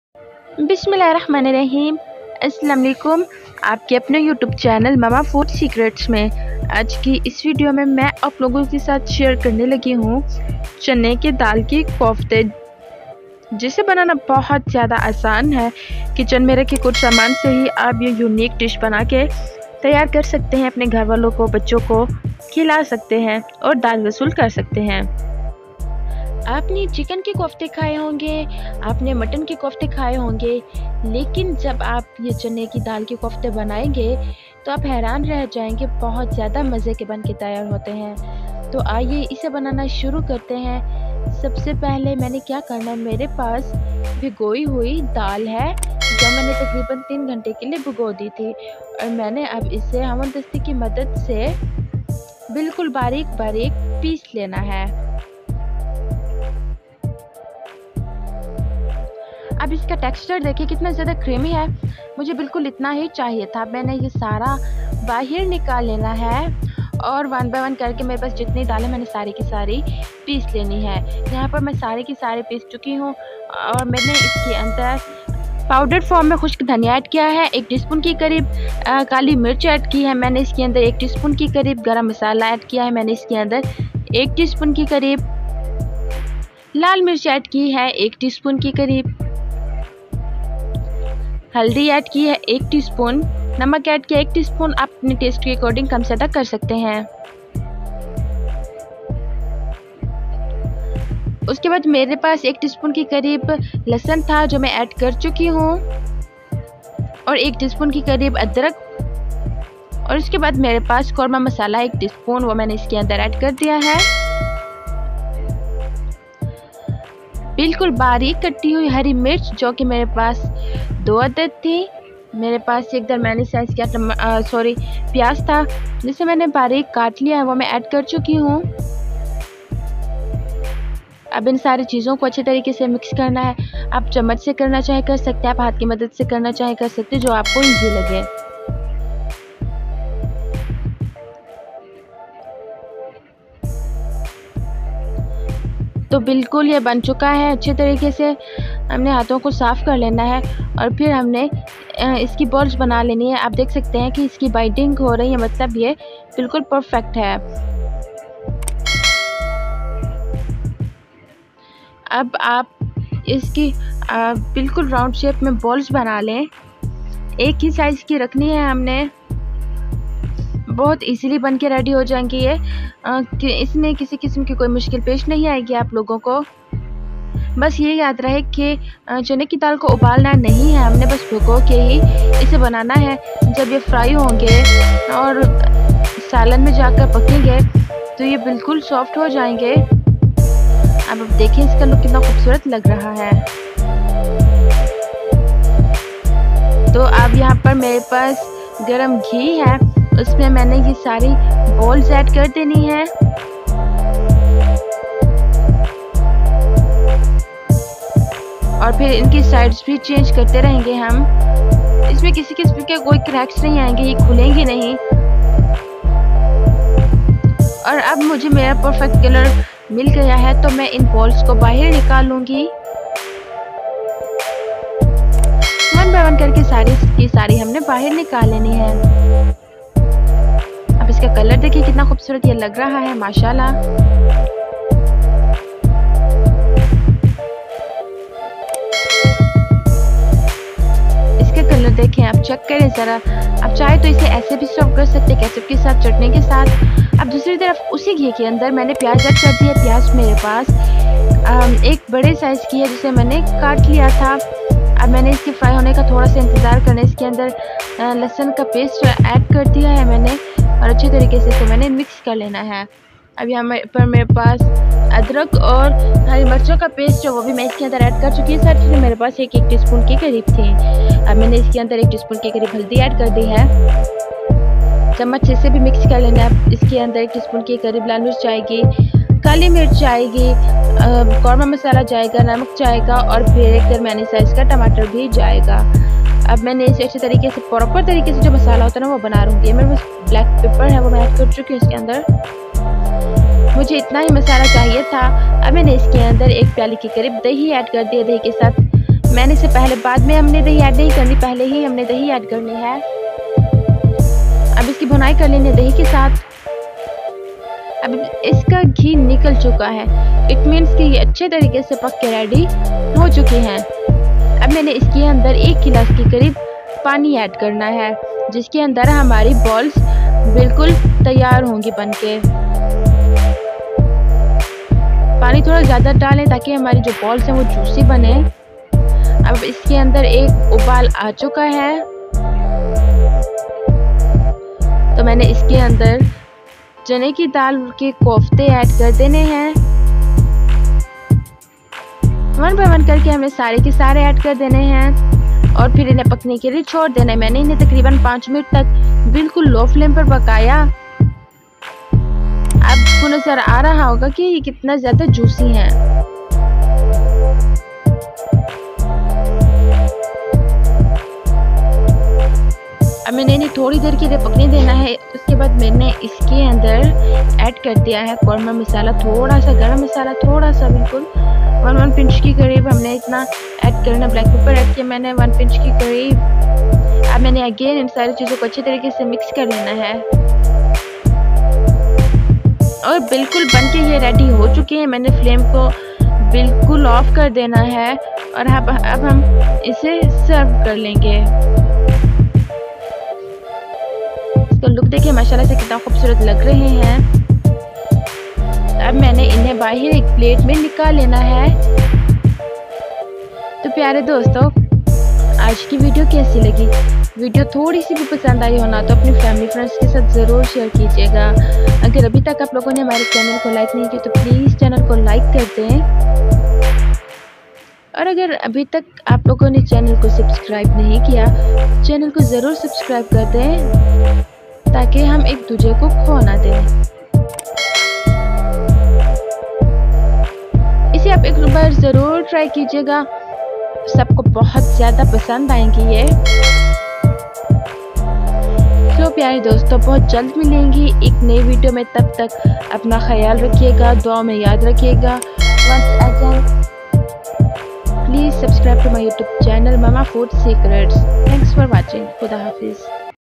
Dal kofta recipe by Mama food's secrets| kofta curry recipe| kofty banany ka tareka
Assalam Alaikum piayry dost aj my apky liy Lai hun chana dal kofta recipe
Kofta curry masala| Masala kofta recipe|